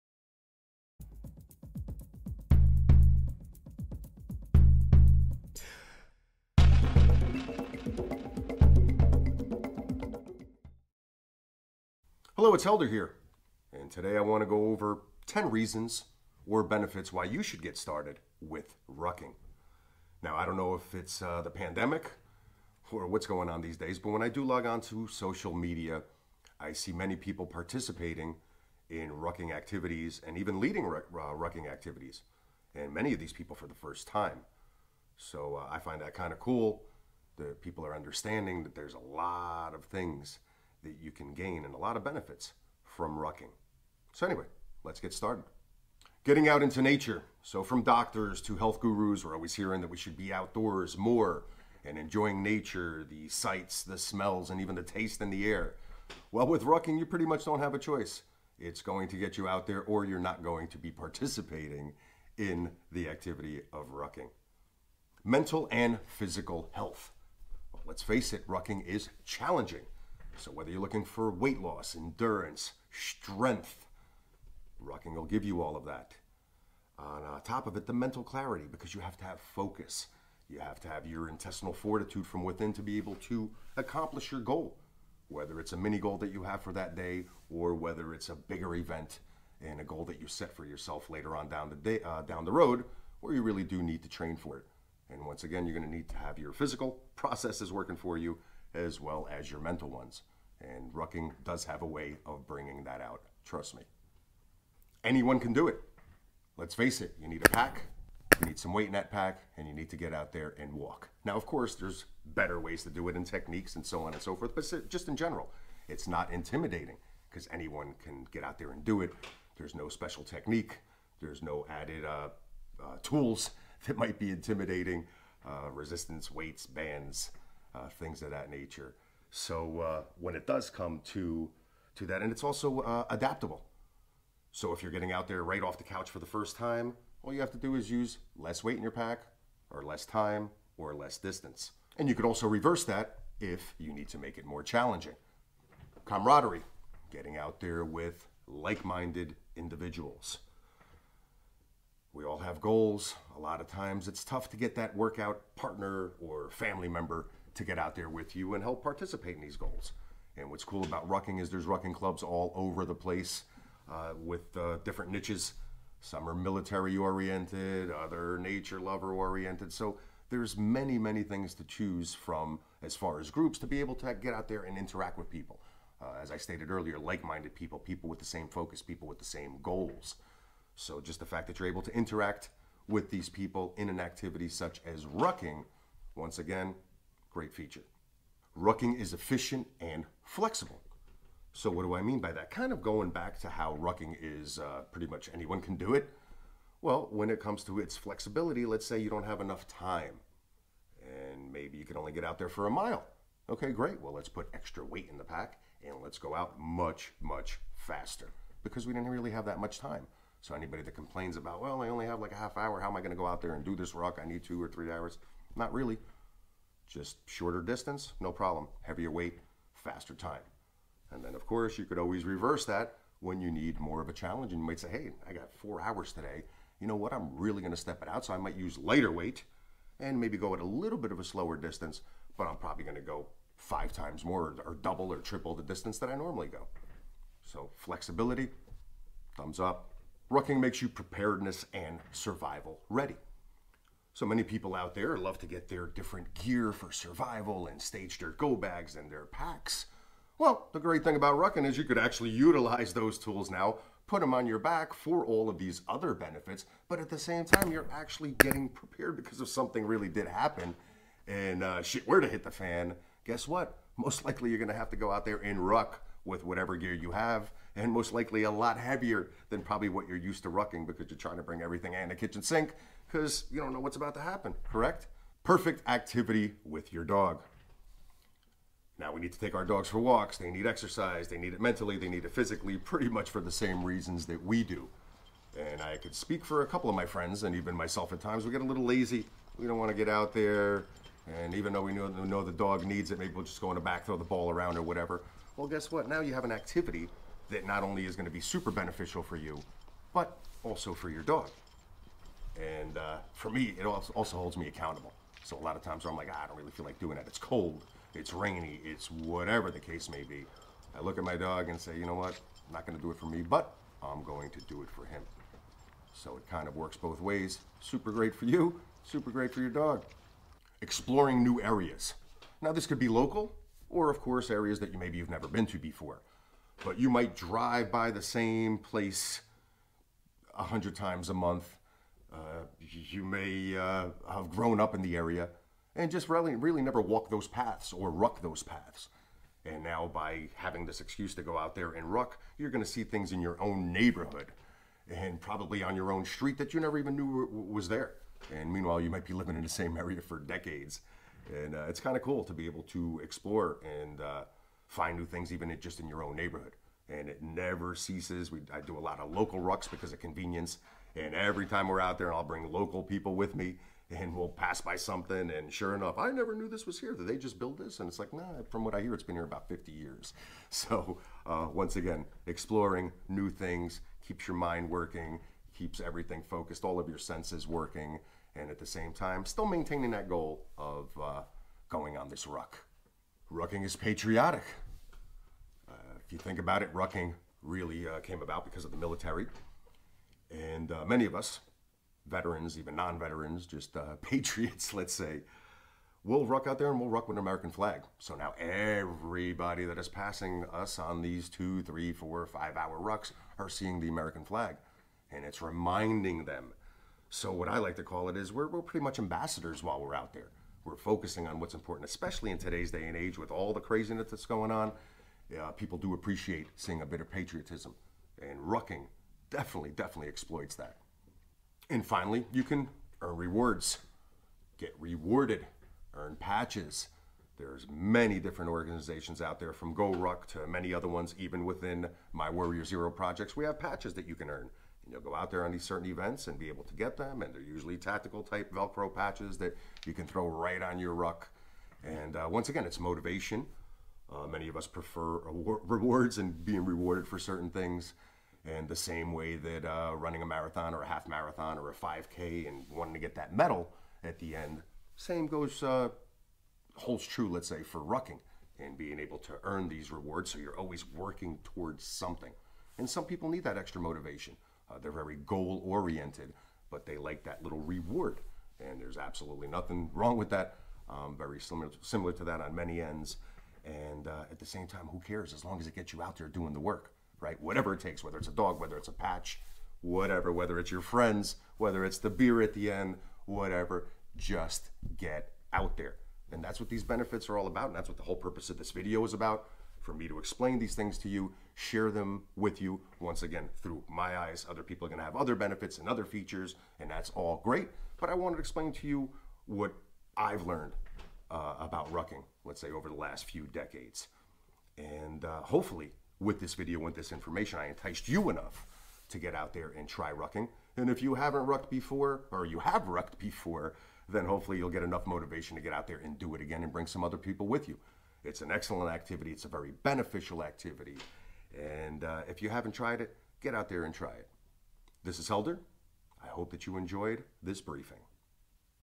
10 Benefits Of Rucking And Why You Should Start Now!
10 Benefits Of Rucking And Why You Should Start Now?
Learn how to ruck by having the essentials. Want to know what to bring on a ruck? Want to learn about essentials that you should have on your ruck? I share rucking tips and rucking techniques for beginners. I share over 10 of rucking benefits and why it is important for you to get started now. Not only a ruck workout but also ruck march training. Make rucking a better experience by having the right gear. Rucking increases weight loss results even when rucking over 40, with injuries. Watch the video by Coach Helder.